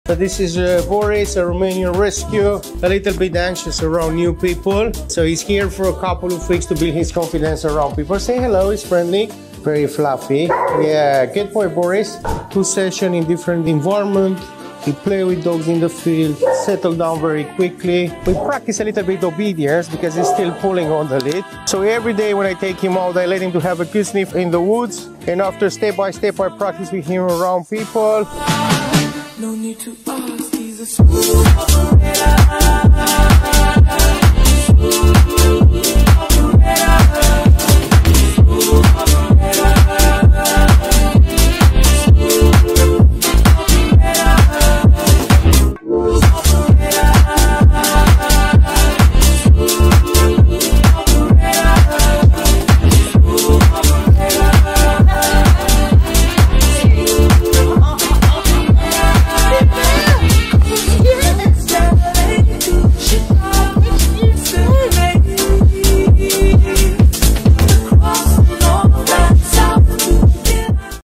How we train rescuedogs at Royvon! Boris Case Study
🤩Take a look at Boris’s time during his ✨Residential Training Programme✨ with RomeoRoyvon 🐶💙

🐶 Boris had a lovely time while working on:
• Anxiety around new people
• Building up his confidence in different environments
• Socialisation
• General Obedience

✨👏🏻You did amazing Boris! Well done👏🏻✨